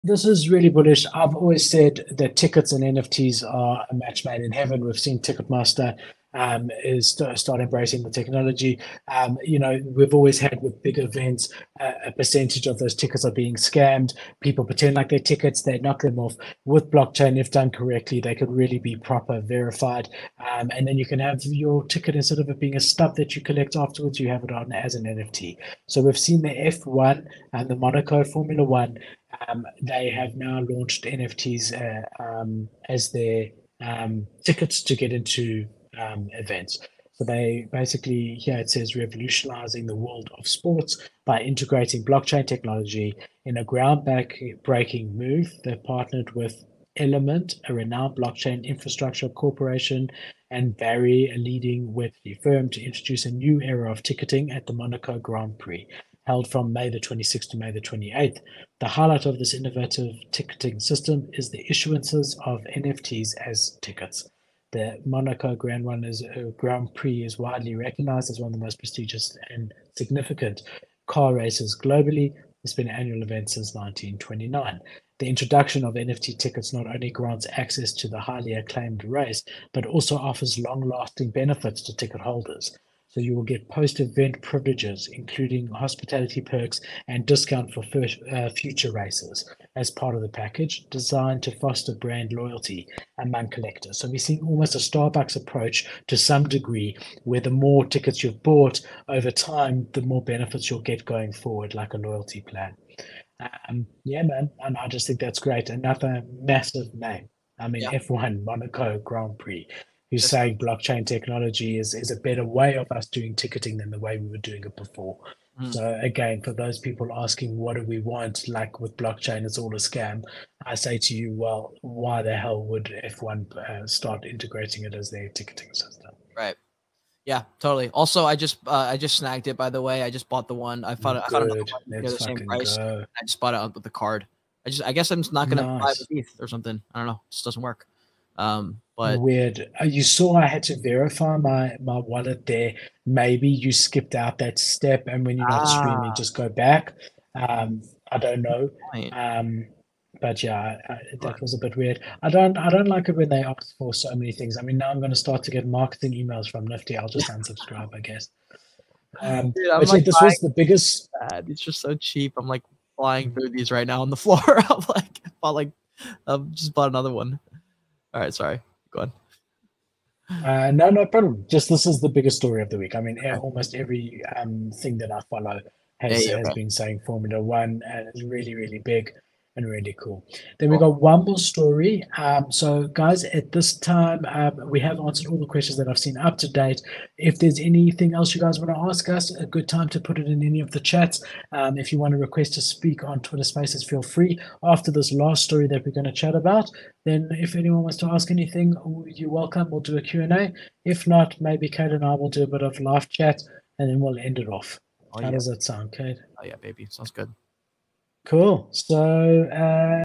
Tickets for the Formula 1 Monaco Grand Prix were issued as NFTs!
Tickets for the Formula 1 Monaco Grand Prix were issued as NFTs minted on Polygon over the weekend, providing fans with future race discounts, hospitality benefits, and enhanced transparency.

. ⭐️THE WEB3 SHOW LINKS⭐️